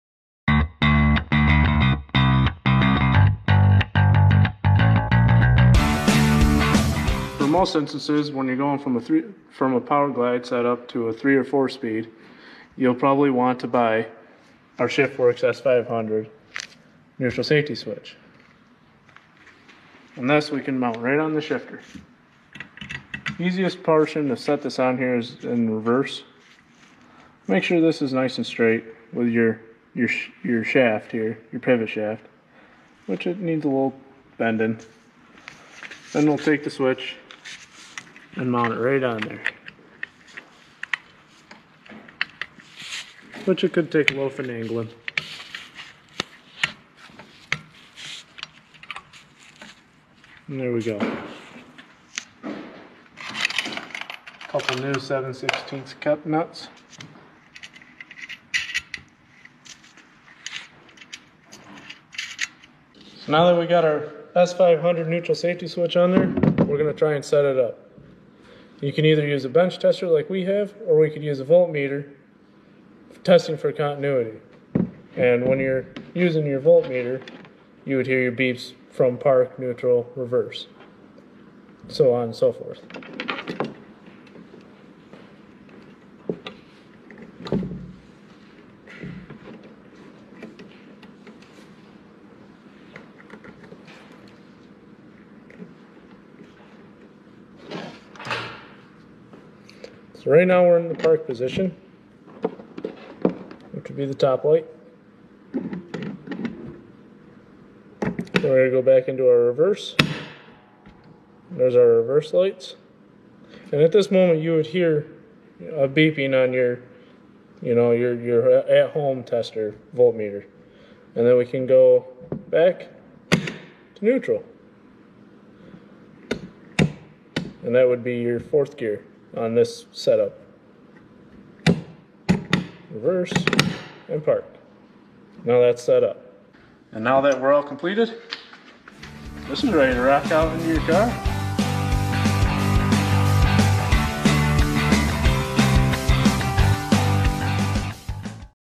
Neutral safety switch installation guide for Chevelle Nova and Impala (1964-1967)
This is an installation guide for Chevelle nova and impala neutral safety switch set up.

 Note this guide will work with a power glide switch too
 But part number S500 is only intended to work with 3 and 4 speed automatic transmissions.